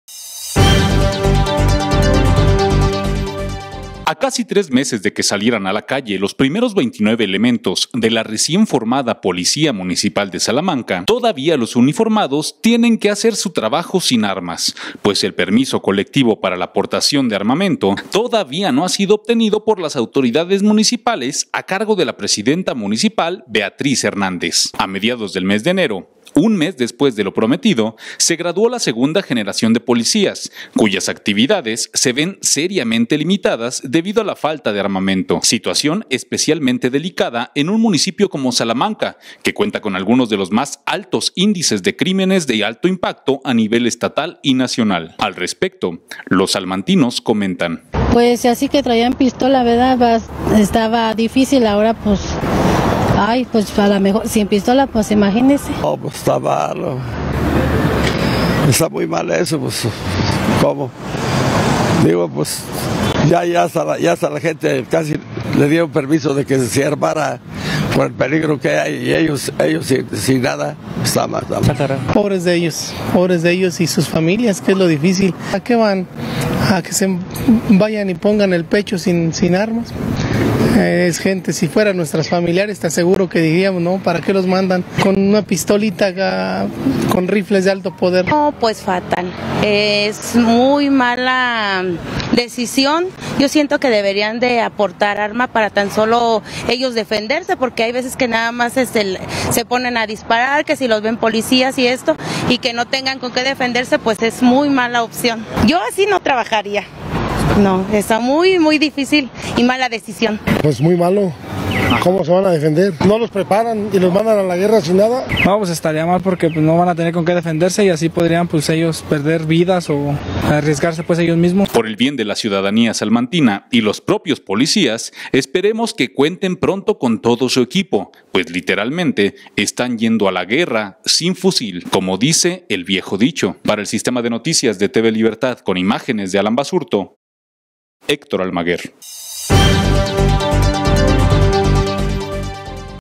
La policía de Salamanca sigue trabajando sin armas
La policía de Salamanca continúa trabajando sin armas, el permiso colectivo no ha podido ser conseguido por el gobierno de Beatriz Hernández a más de 3 meses que se graduaran los primeros elementos.

A casi 3 meses de que salieran a la calle los primeros 29 elementos de la recién formada policía municipal de Salamanca todavía los uniformados tienen que hacer su trabajo sin armas, pues el permiso colectivo para la portación de armas todavía no ha sido obtenido por las autoridades municipales a cargo de la presidenta municipal Beatriz Hernández.

A mediados del mes de enero, 1 mes después de lo prometido se graduó la segunda generación de policías cuyas actividades se ven seriamente limitadas debido a la falta de armamento.

Situación especialmente delicada en un municipio como Salamanca, que cuenta con algunos de los más altos índices de crímenes de alto impacto a nivel estatal y nacional.